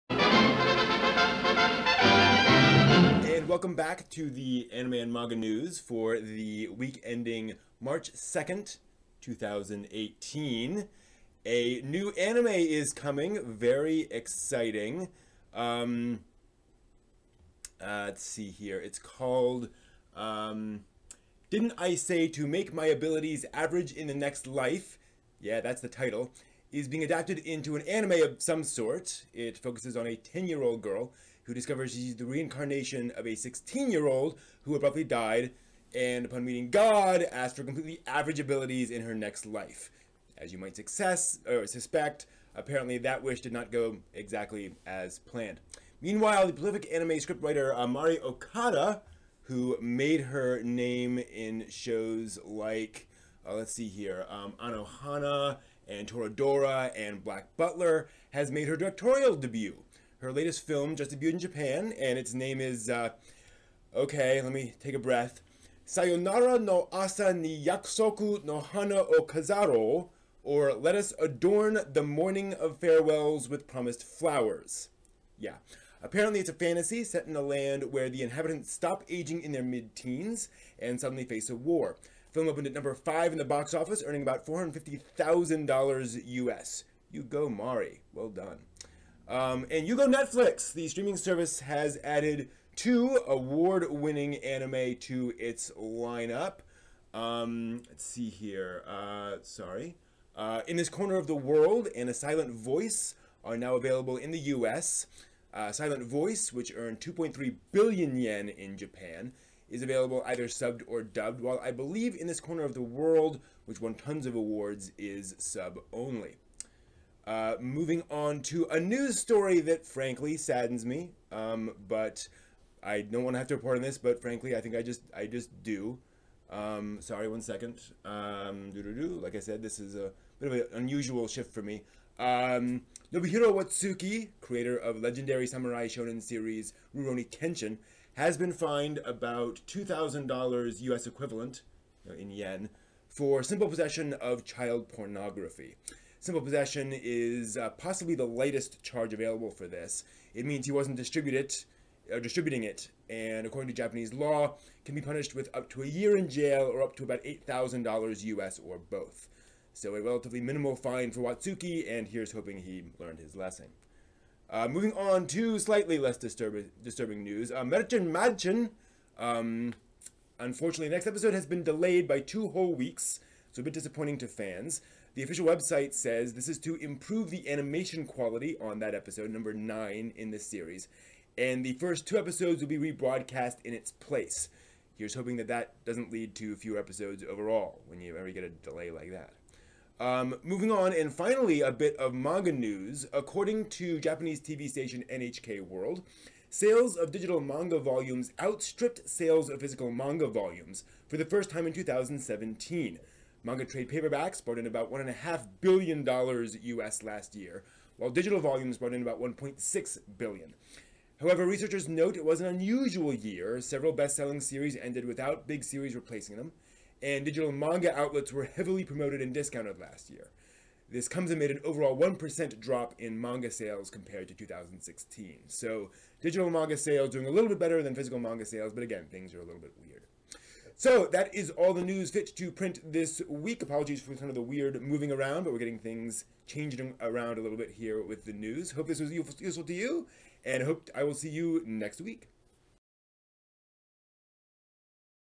News: New Anime Announced! Mari Okada's First Film Released! 2 Big Anime Films on Netflix!
Thanks as always to ANN for the headlines.